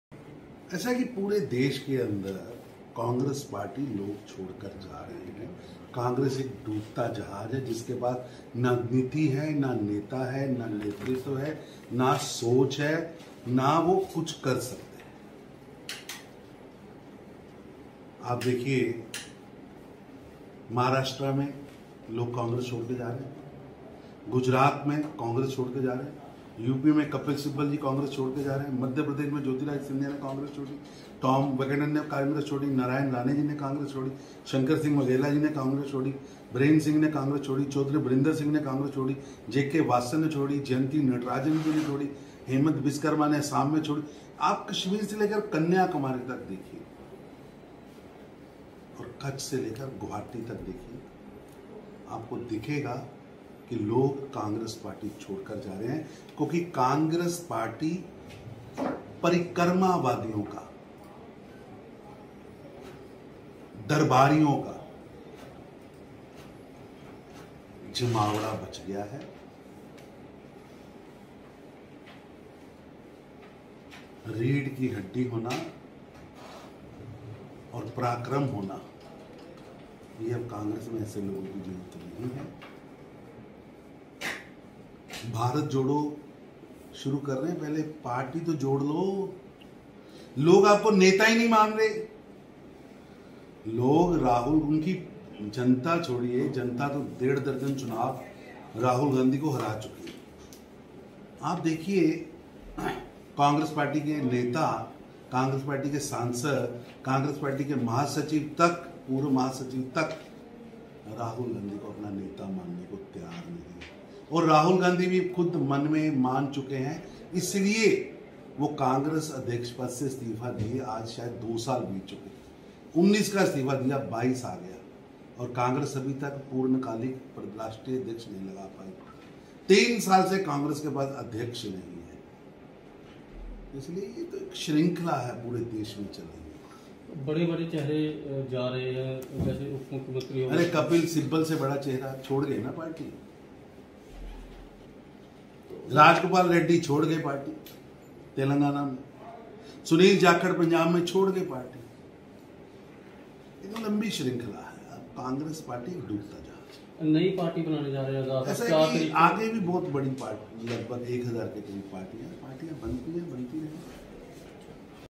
Congress a sinking ship; from Kashmir to Kanyakumari, from Kutch to Guwahati
Congress a sinking ship; from Kashmir to Kanyakumari, from Kutch to Guwahati -- all leaders are quitting congress party: Tarun Chugh